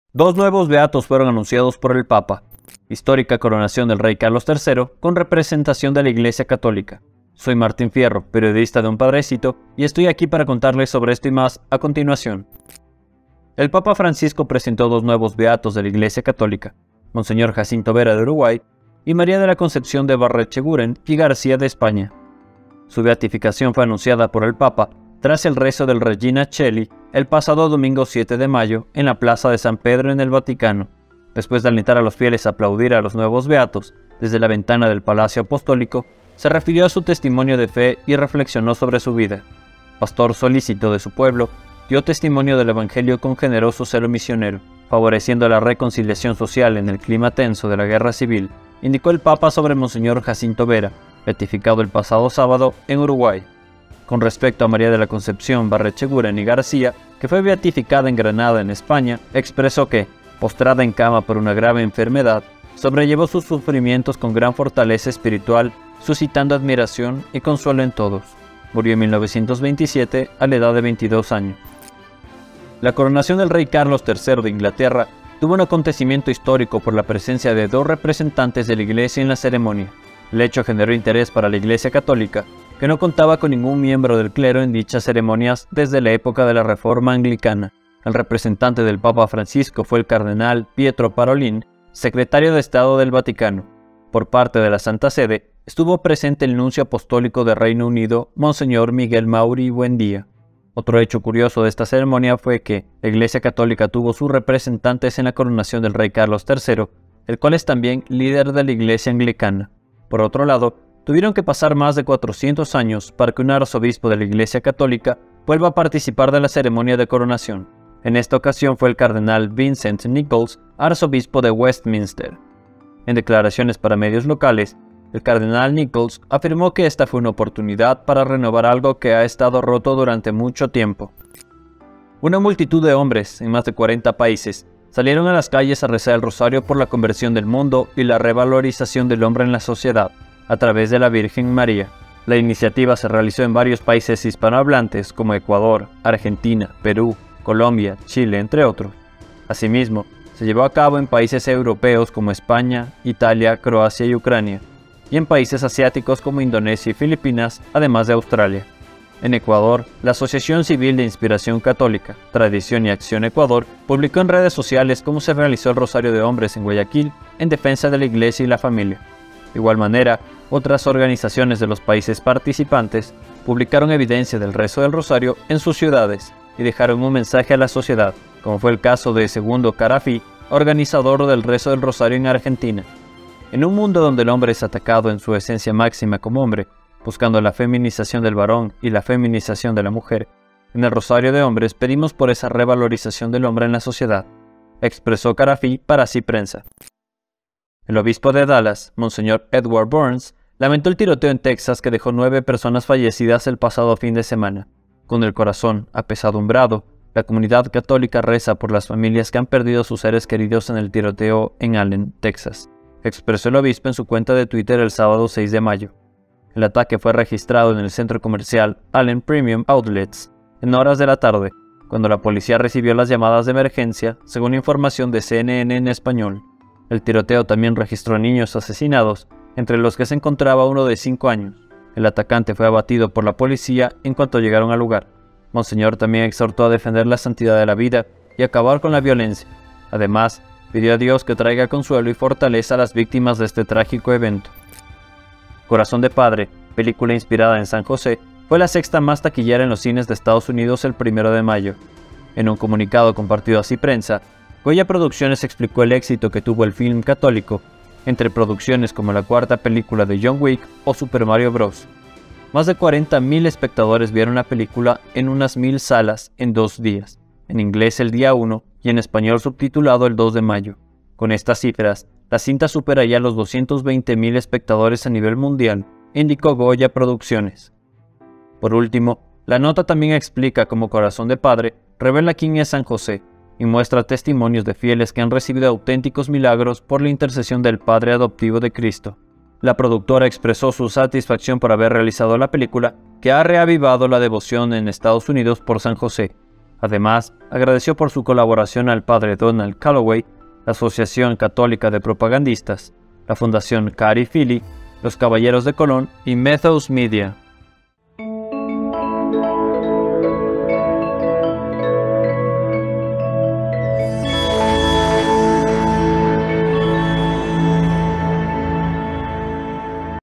🔺Noticias Católicas 10/5👉 Corazón de padre. Nuevos Beatos. Coronación Rey Carlos III.
🔺Noticias Católicas 10/5👉 Corazón de padre. Nuevos Beatos. Coronación Rey Carlos III. Rosario de hombres.